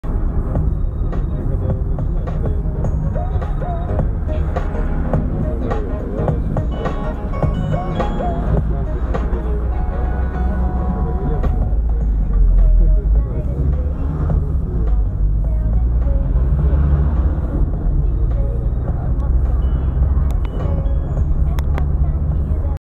Нарушение ПДД? Daewoo AX3403AP
Нарушение ПДД? Daewoo  AX3403AP - TAXI -
Харьков пр-т. Гагарина